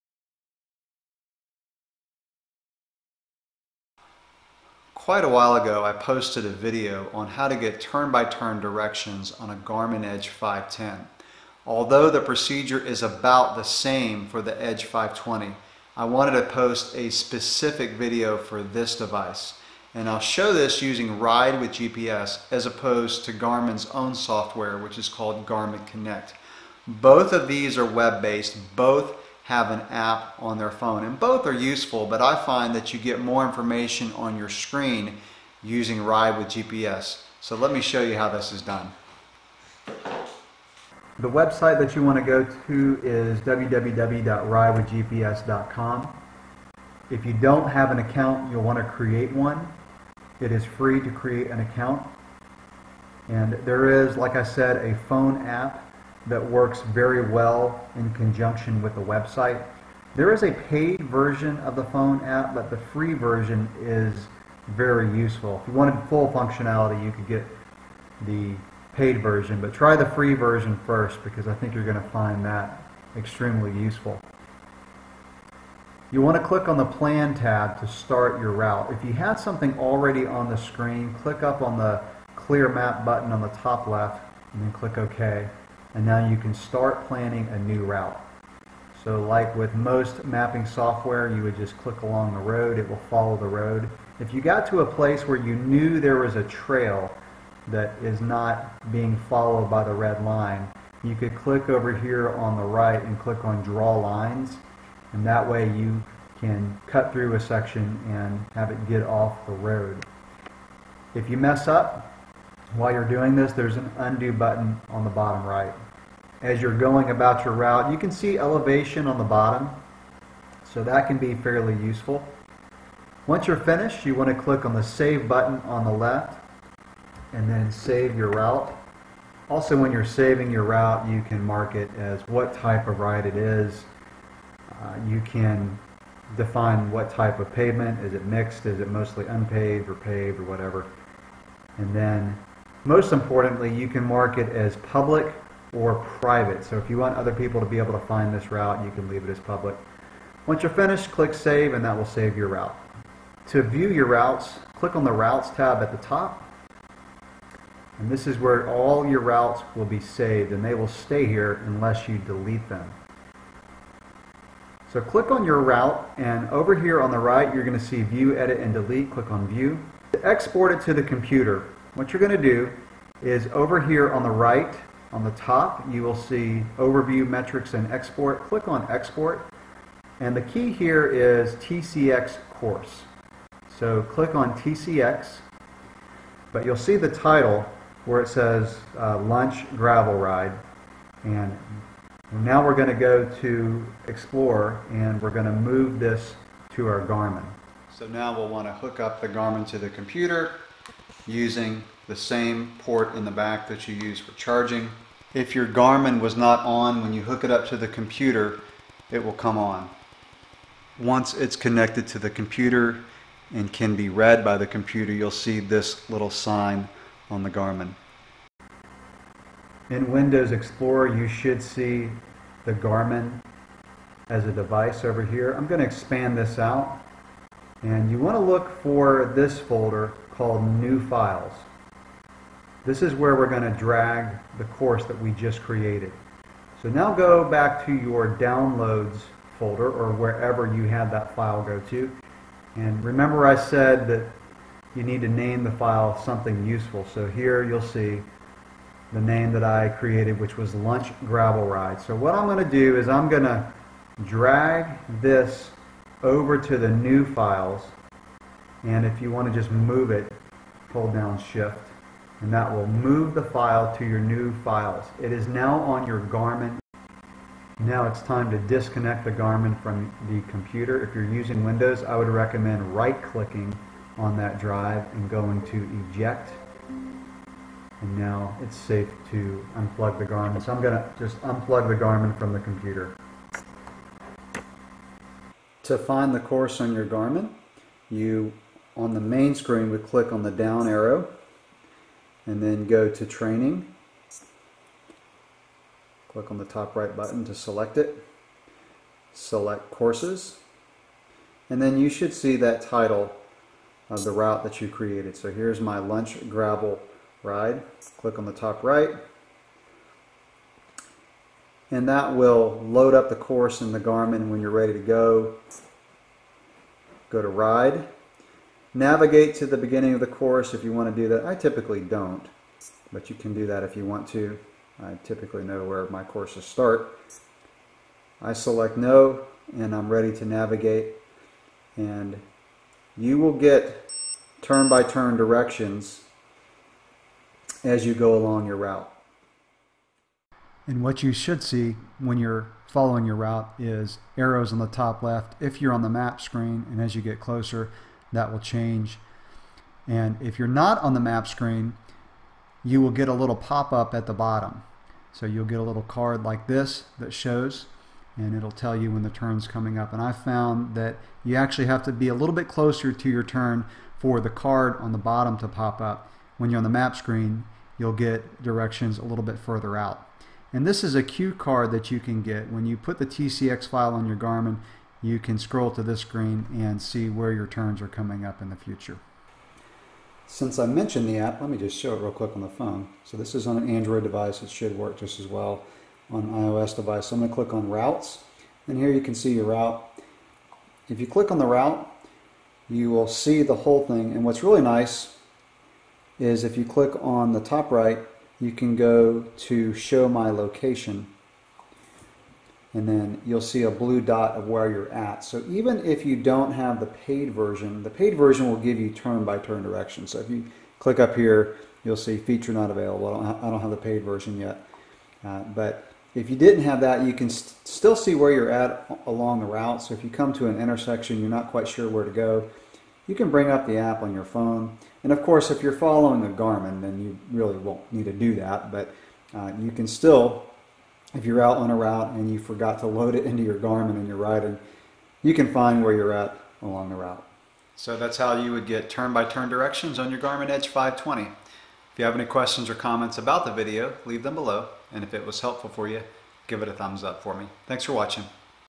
Garmin Edge 520 Turn Direction Using Ride With GPS
This video is a tutorial of how to get turn by turn directions on the Garmin Edge 520 cycling computer.  I show this using Ride With GPS.